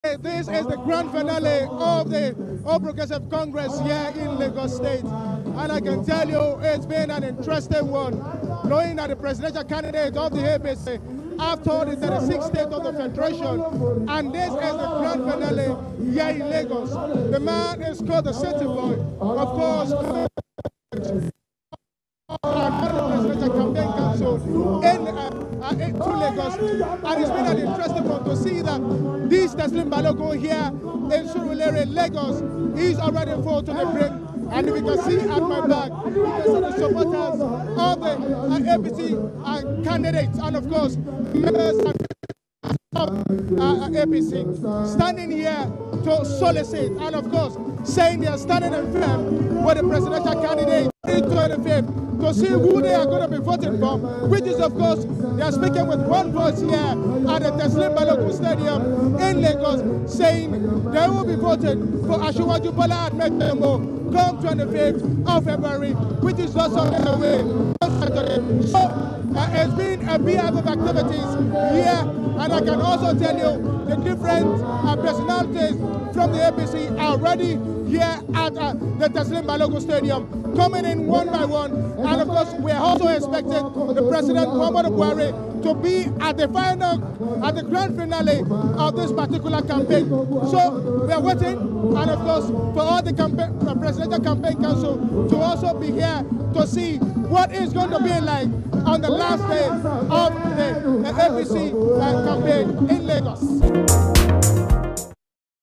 POLITICAL UPDATE: APC Holds Campaign Rally at Teslim Balogun Stadium, Lagos State Nigeria
APC Holds Campaign Rally at Teslim Balogun Stadium, Lagos State Nigeria